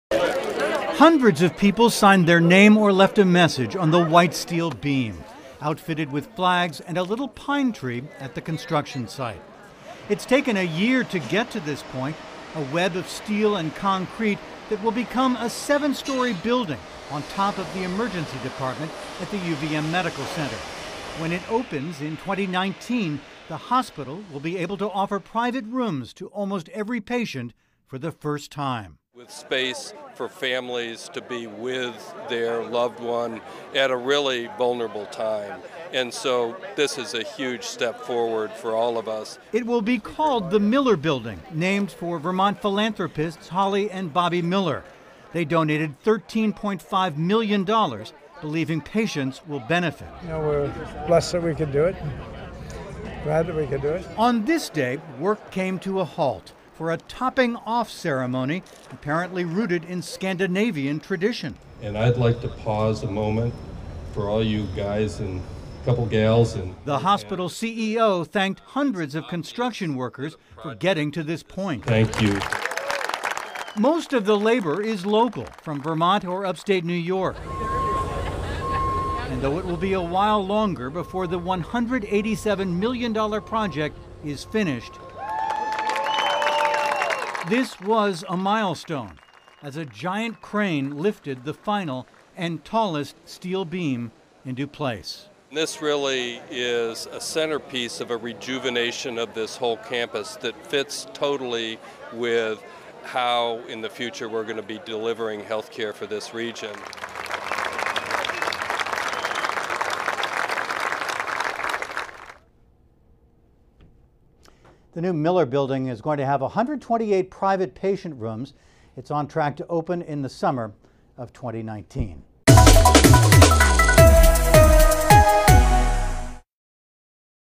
UVM Medical Center marks construction milestone
A major addition to the University of Vermont Medical Center campus in Burlington is on track to open in 2019.